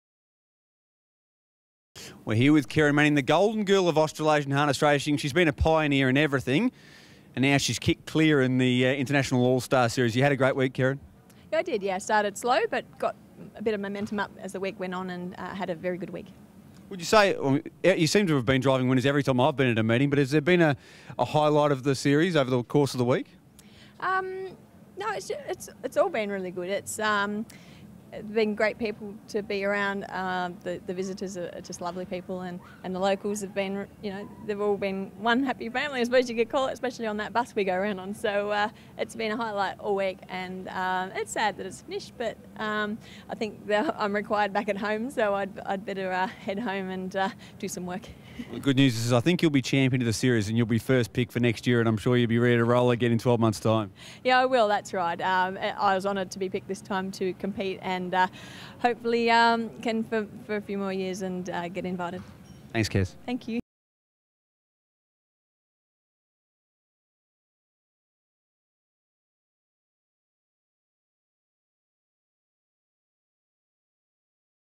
TrotsAllStars: Kerry Manning
International All Star Series winner Kerryn Manning reflects on a victorious ‪#‎TrotsAllStars‬ campaign, which she says has been "a highlight all week and it’s sad that it’s finished". Read the wrap of her magnificent week